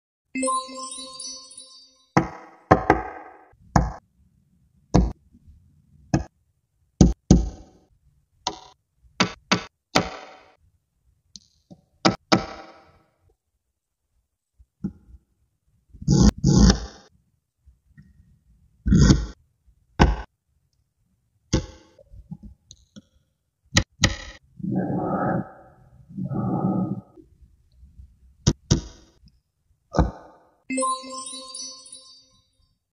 🌈 🍧 🍭 Perfect Rainbow Ice Cream From Magnetic Balls
Best Magnet Art🌈🍧Perfect Rainbow Ice Cream From Magnetic Balls 🍭 Tiny Magnetic Balls | My Magnet

make/build with Magnetic Balls, please tell us, we will try to make it

A handicraft product with magnetism, I hope you will enjoy videos with asmr relax and satisfying video~